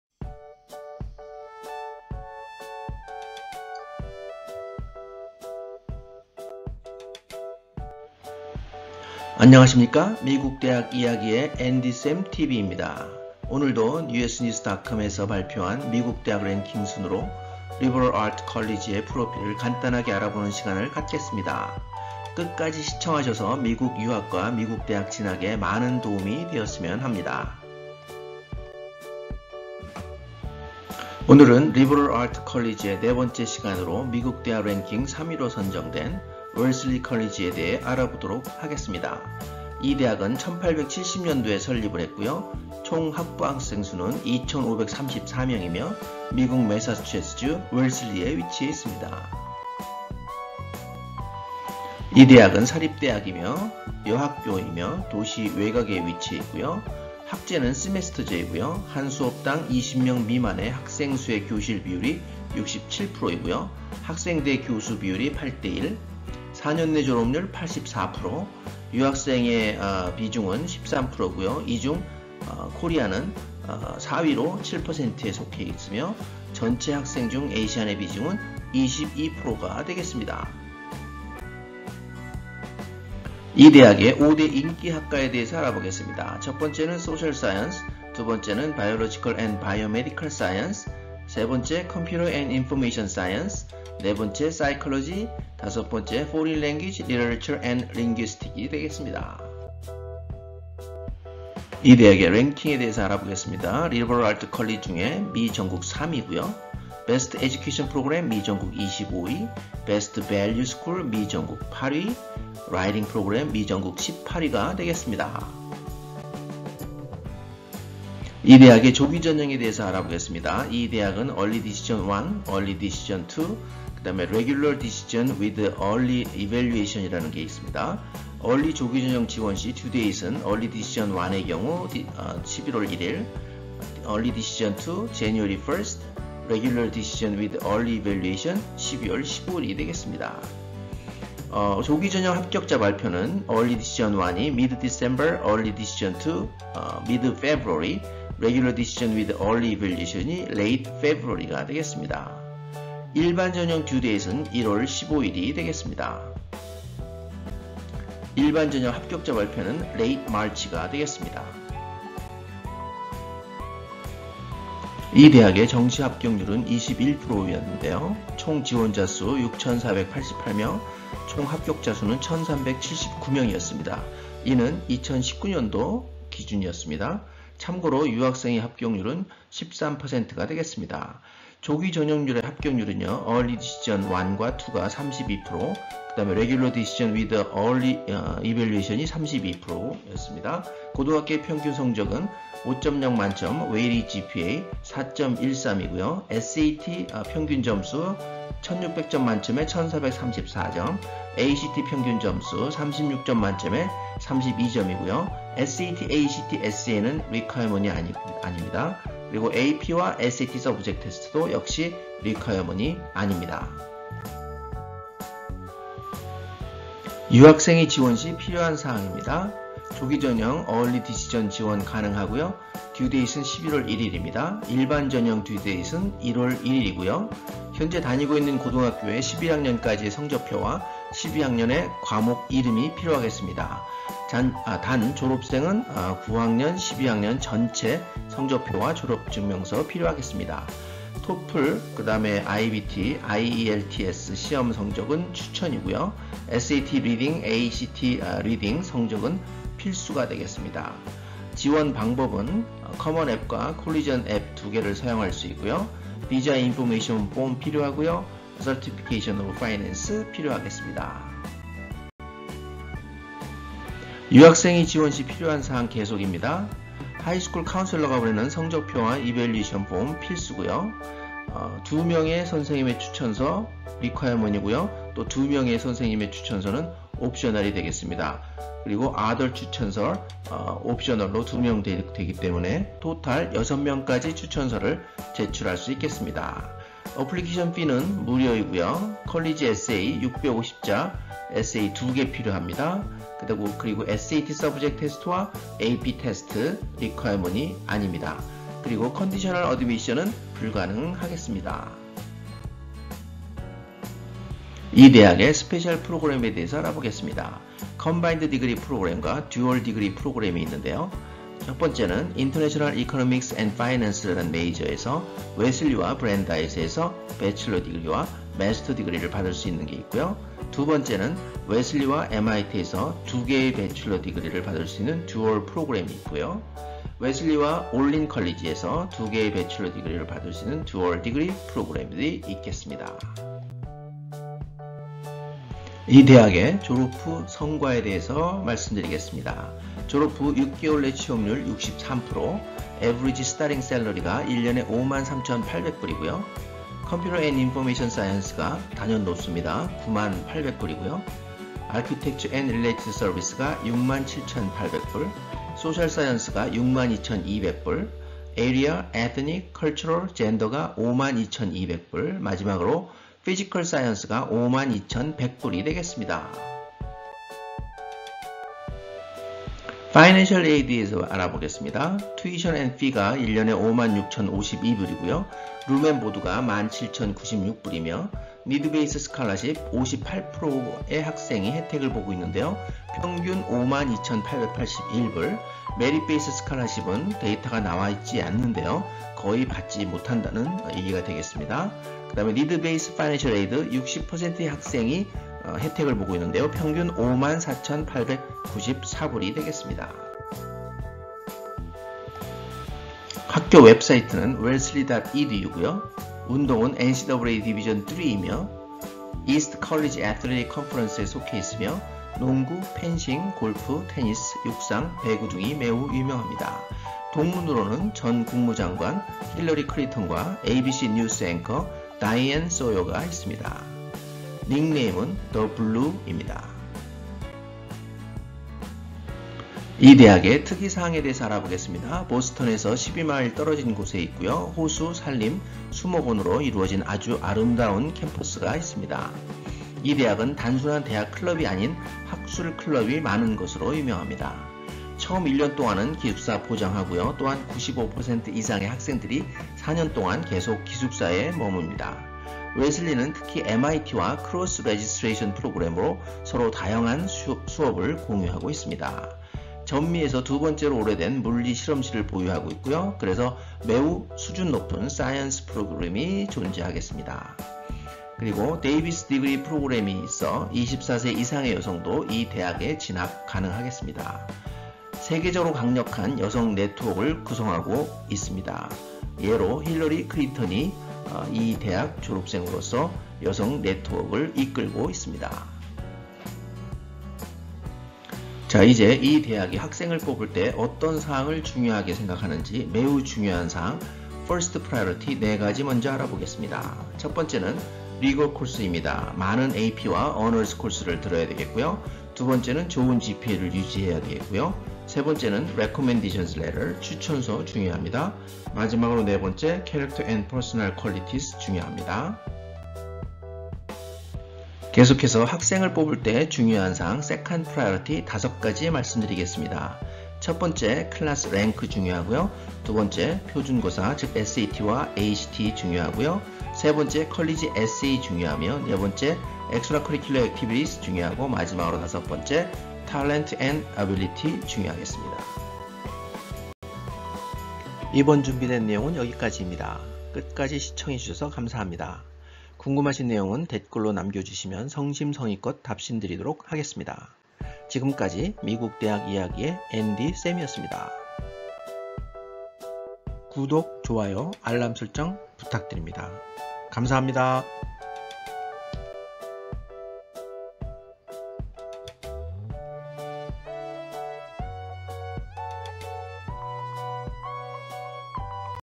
[엔디쌤tv : 미국 대학 이야기] 64 번째 - 웰즐리 대학교 (Wellesley College: MA)
안녕하세요? 이번에는 usnews.com 에서, 미전국 대학 랭킹 3위(Liberal Arts College 부분)에 선정된 Wellesley College (웹사이트: wellesley.edu)에 대한 영상을 올려드립니다. 끝까지 시청 부탁 드리며, 문의 사항이 있으시면 댓글도 부탁 드립니다. 또한, 카타고리별로도 정리해 놓았으니, 시간이 되시면, 재생 목록에서도 시청하실것을 추천 드립니다. 감사합니다.